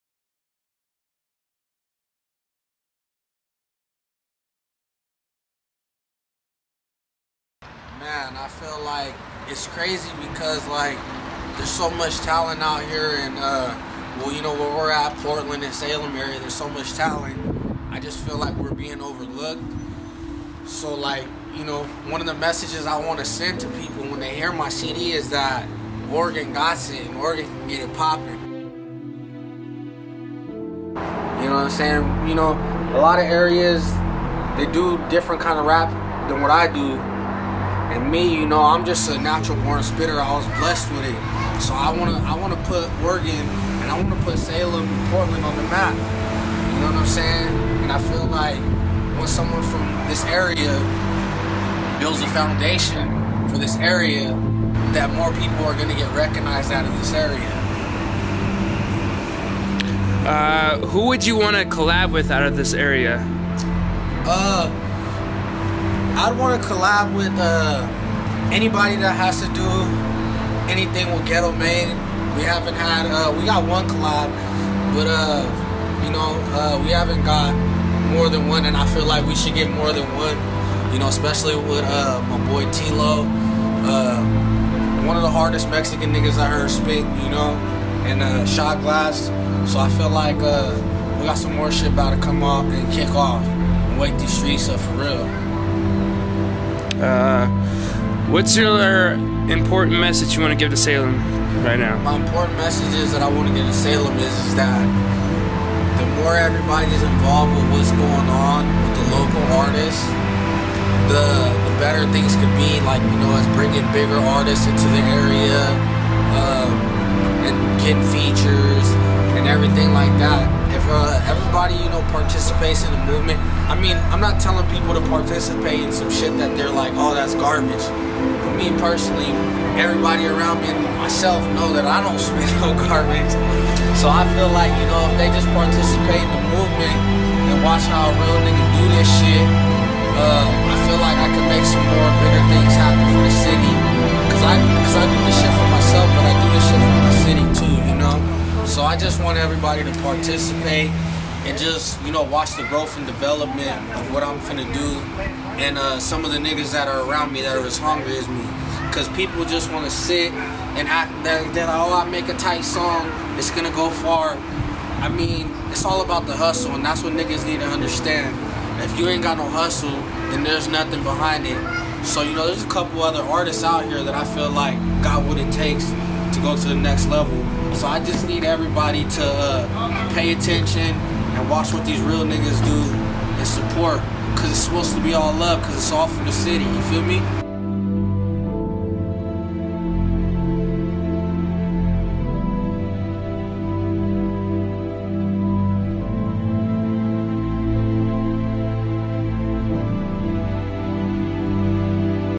Collective Films - Jose Cuervoo - Interview Series Part 1
Talking to Jose Cuervoo about some view on Salem and upcoming project